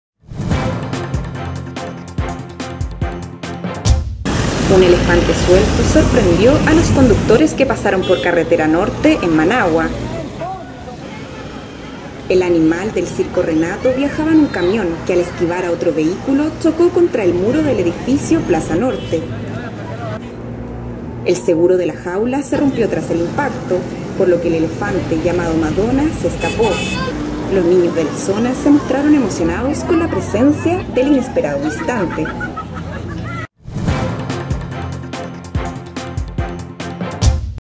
Elefante suelto en Managua
Un camión que transportaba a un elefante tuvo un accidente por lo que el animal estuvo suelto en Carretera Norte, Managua.